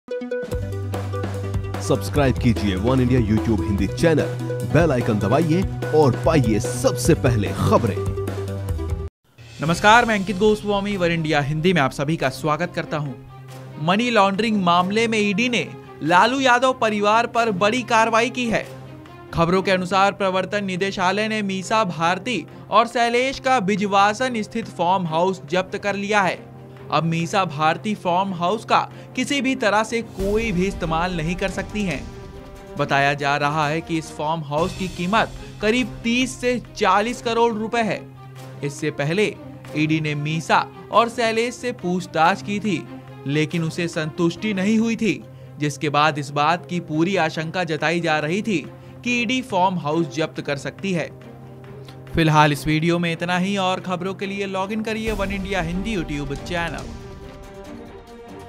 Misa Bharti farmhouse in Delhi attach by ED | वनइंडिया हिंदी
The farmhouse located in south Delhi's Bijwasan area was sealed by Enforcement Directorate sleuths in connection with a money laundering case against Lalu Yadav's daughter Misa Bharti. Watch this video.